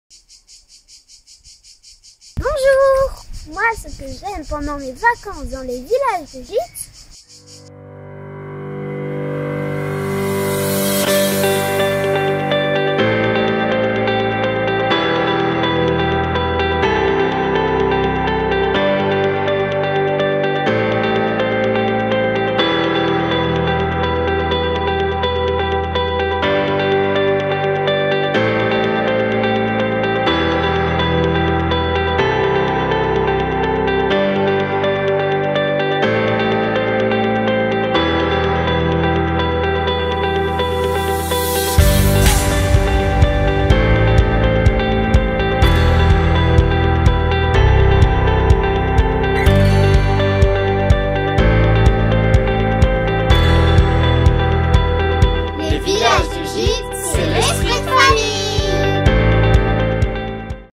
Vidéo ambassadeur village de gîtes Le Clos du Moulin - Dordogne
Vidéo vacances concours 2017 : Merci à la Famille Guiraud pour cette vidéo de leur vacances au village de gîtes le Clos du Moulin en dordogne dans le cadre du concours mesvacancesenvillagesdegites.